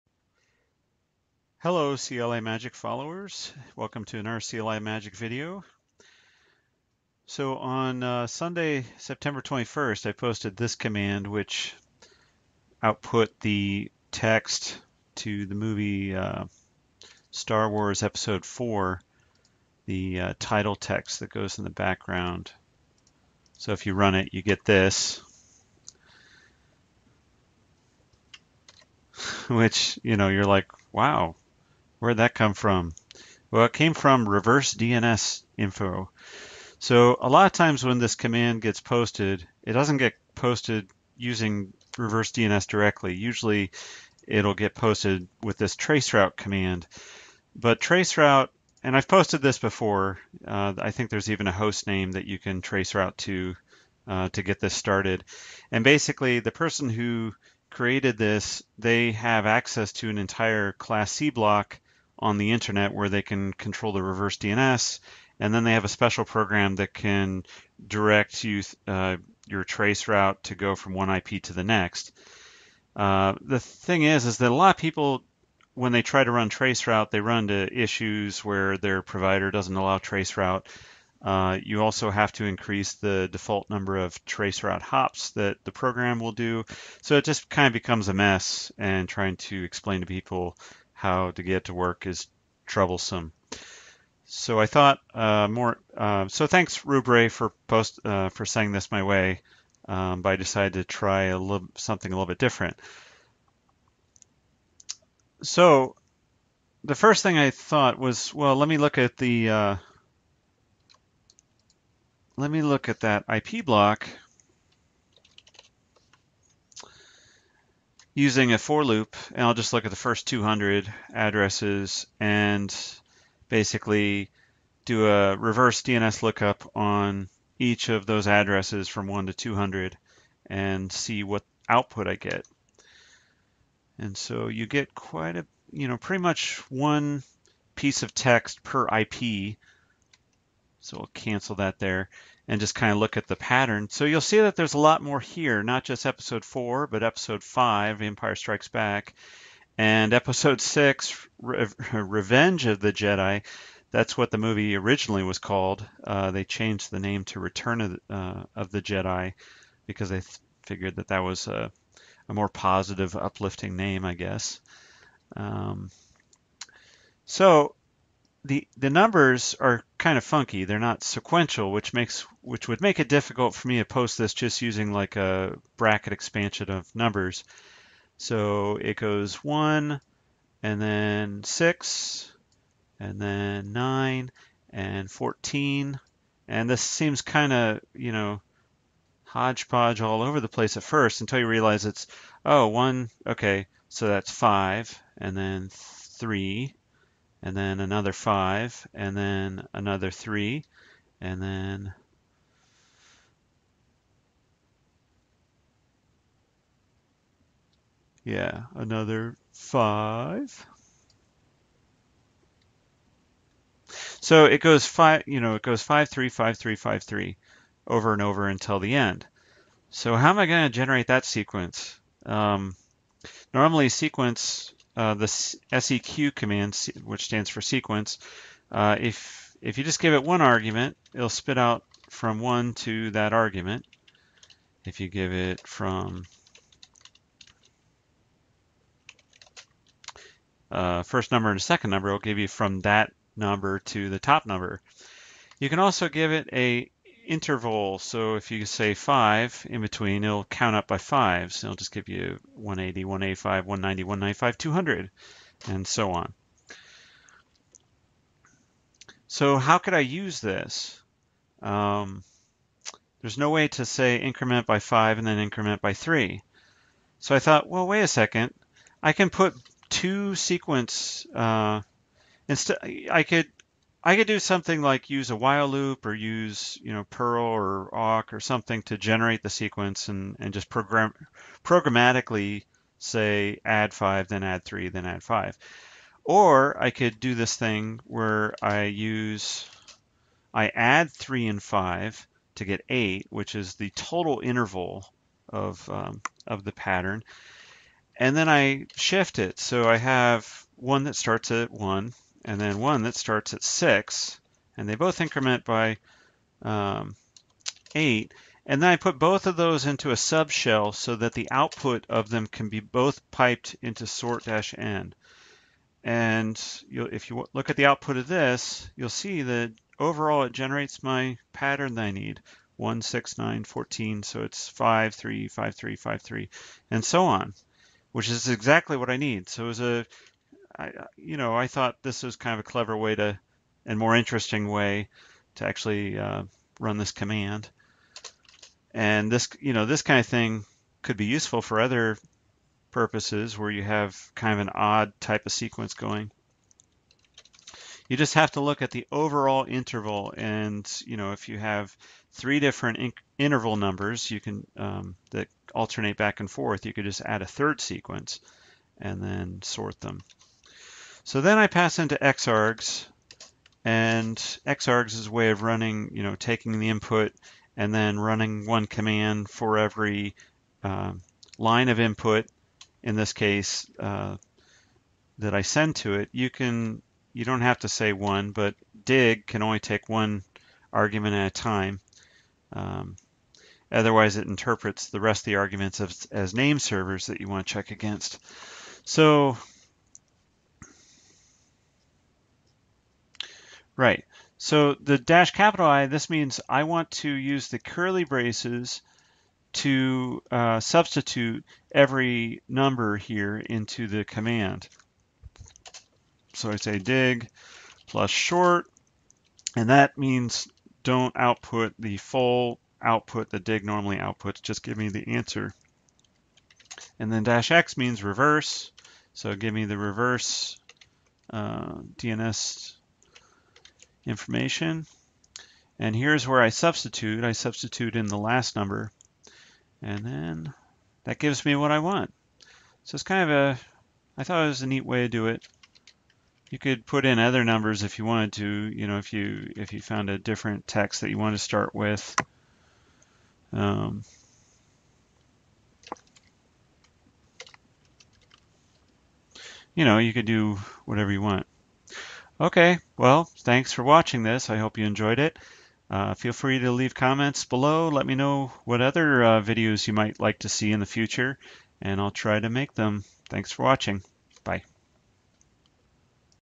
Star Wars Reverse DNS sequence
Often times people post a traceroute command that generates this text, which comes from reverse DNS. I decided to skip the middle man and go straight for a command that gets the reverse DNS directly and in the mean time I found a novel way to generate mixed sequences.